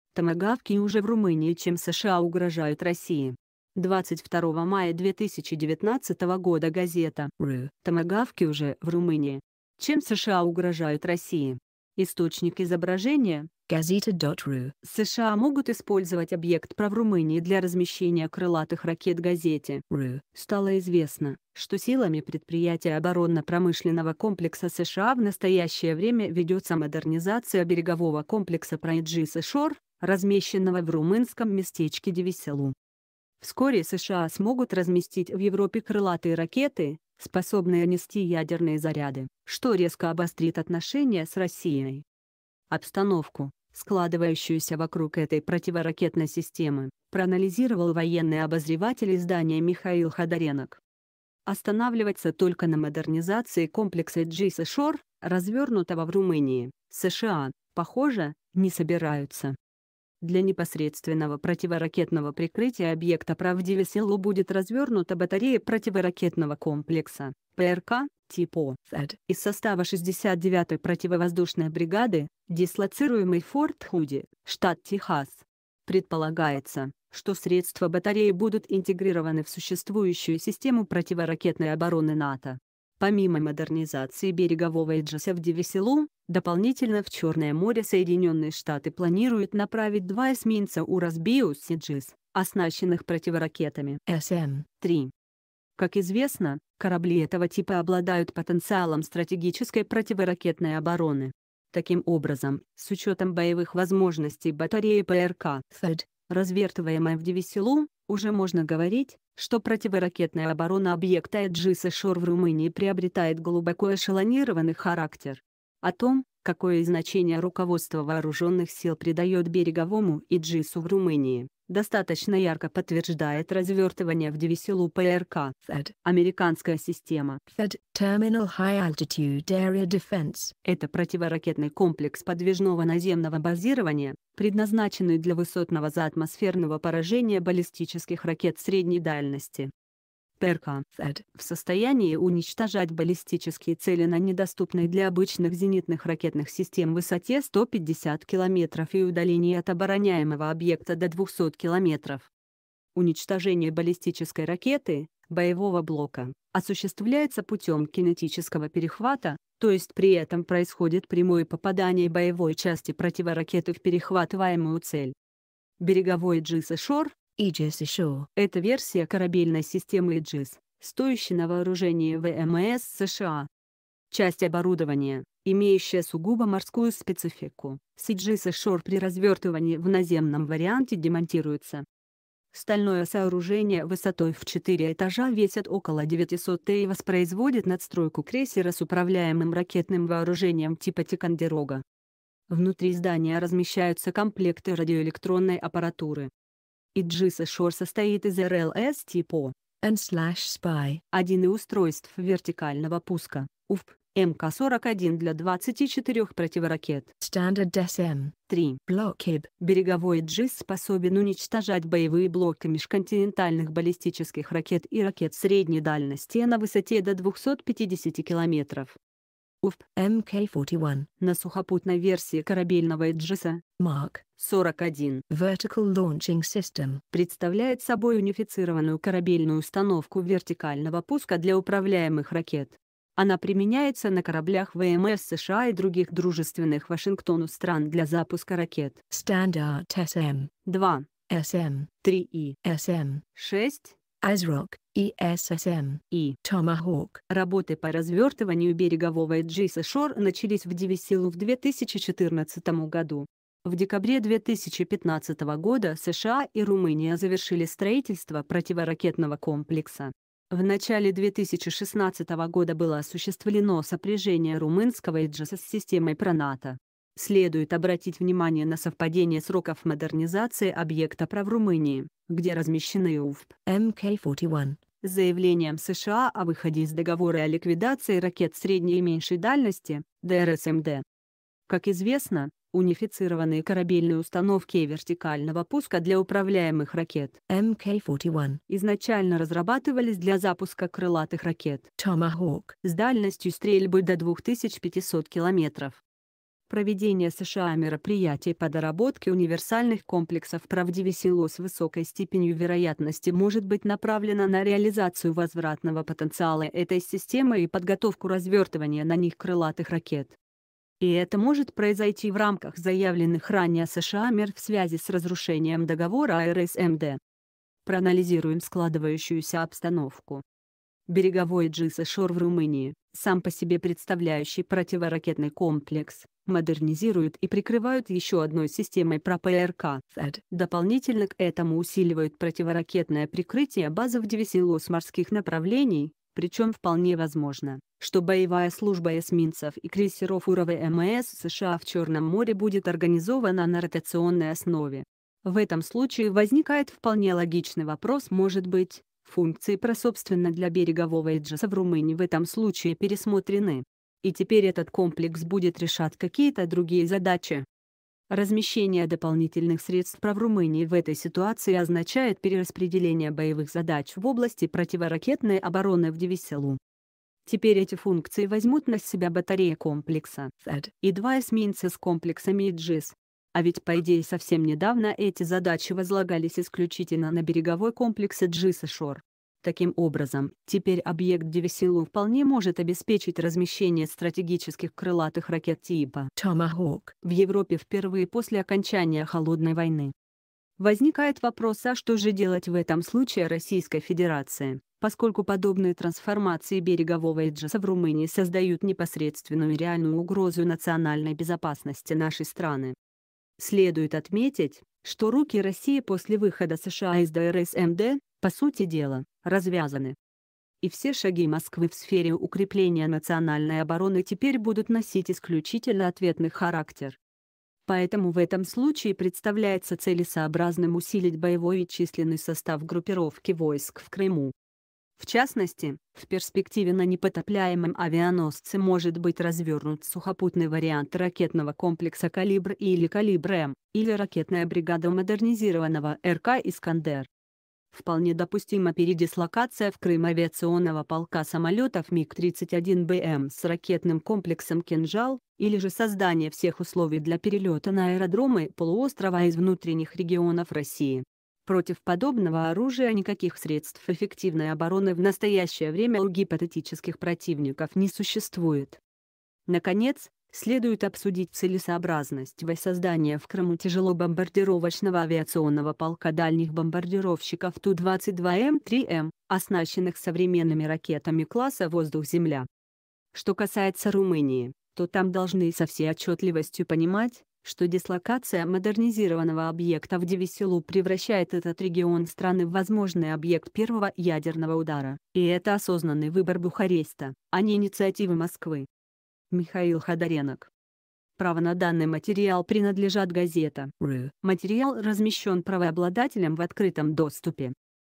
Томагавки уже в Румынии Чем США угрожают России
Томагавки уже в Румынии Чем США угрожают России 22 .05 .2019 Газета .ru Томагавки уже в Румынии Чем США угрожают России Источник изображения gazeta .ru США могут использовать объект ПРО в Румынии для размещения крылатых ракет Газете .Ru стало известно, что силами предприятий оборонно-промышленного комплекса США в настоящее время ведется модернизация берегового комплекса ПРО Иджис Эшор , размещенного в румынском местечке Девеселу . Вскоре США смогут разместить в Европе крылатые ракеты, способные